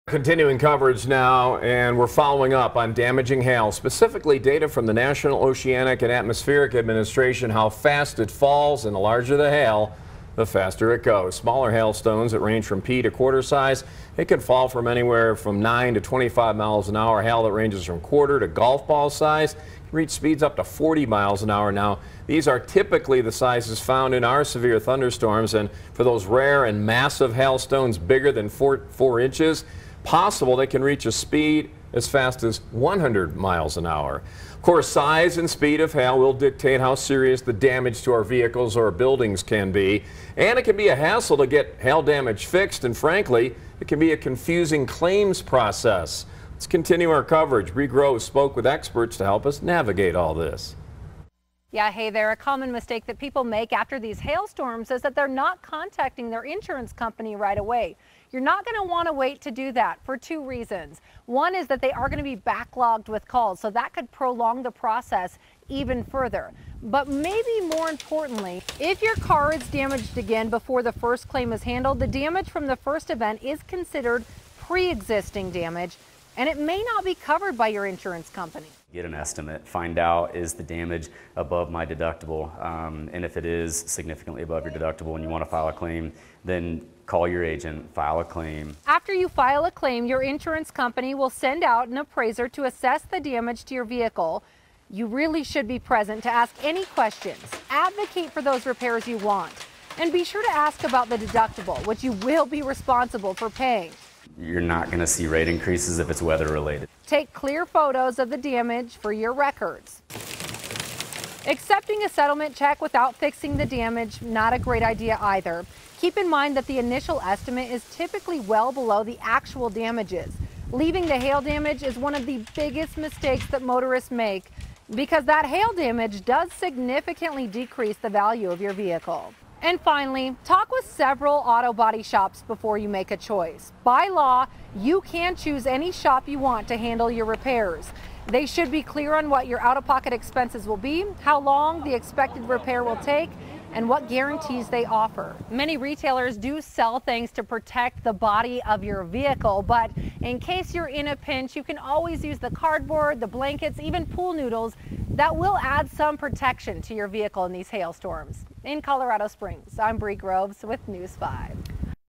Navigating hail damage insurance claims
Size and speed of hail will dictate how serious the damage to our vehicles and buildings. And it can be a hassle to get hail damage fixed, and frankly, it can be a confusing claims process. Our Brie Groves spoke with the experts to help you navigate the process.